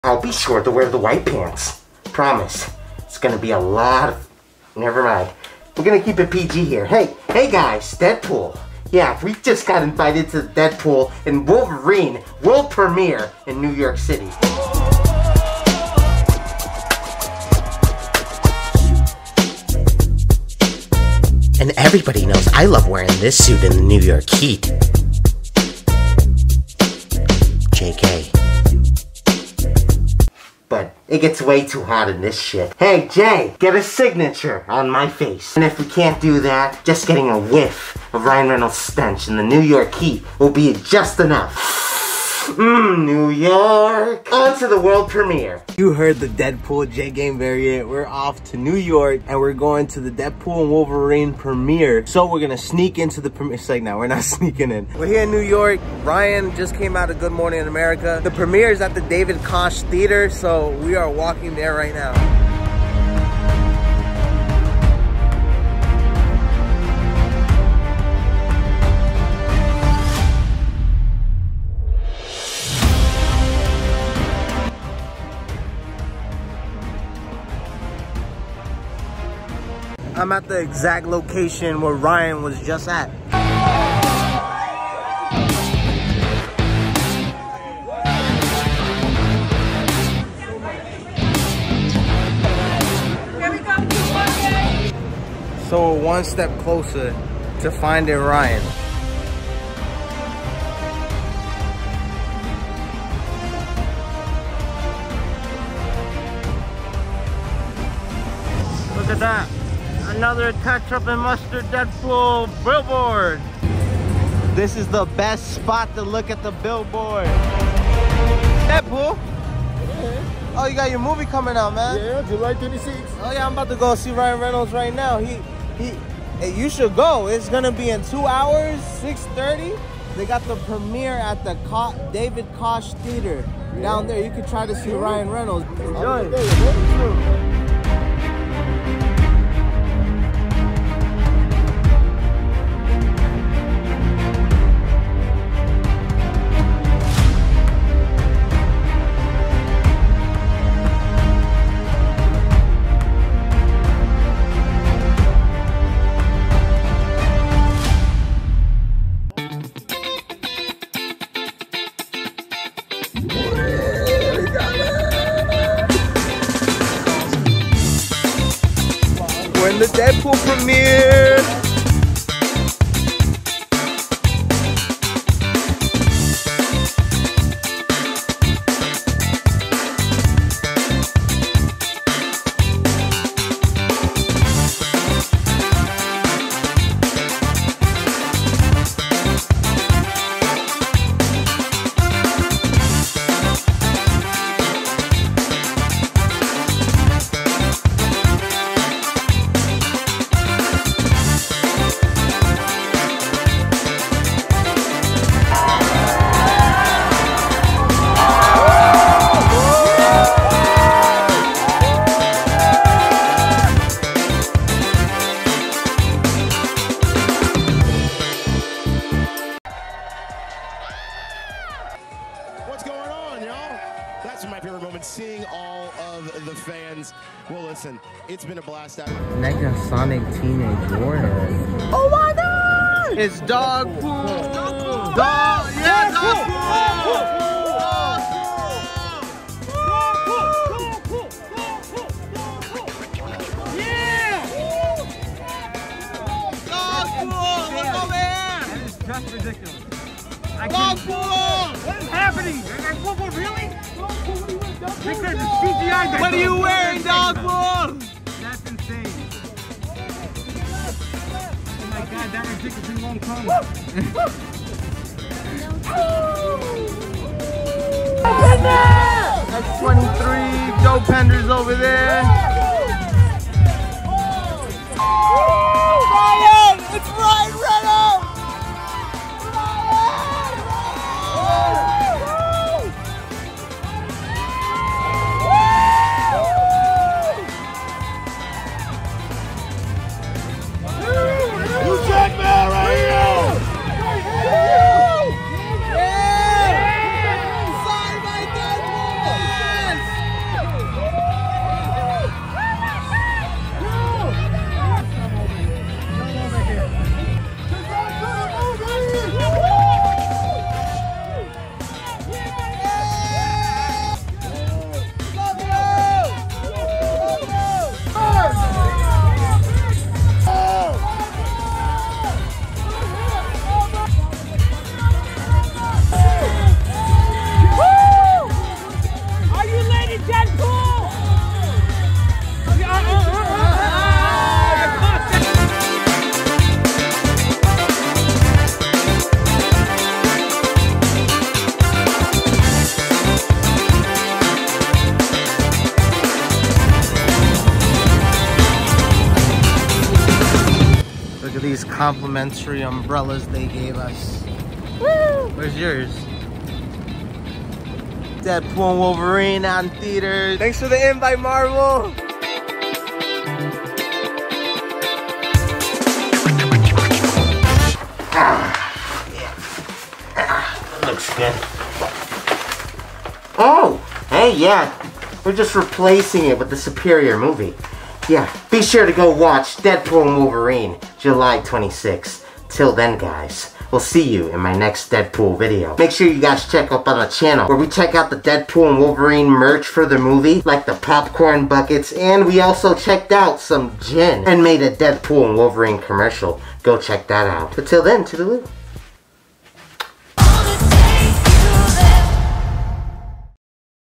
Meeting Ryan Reynolds & Hugh Jackman on the Red Carpet! Deadpool & Wolverine World Premiere NYC Vlog
I got to see Ryanreynolds at the WORLD PREMIERE OF DeadpoolandWolverine. ENJOY!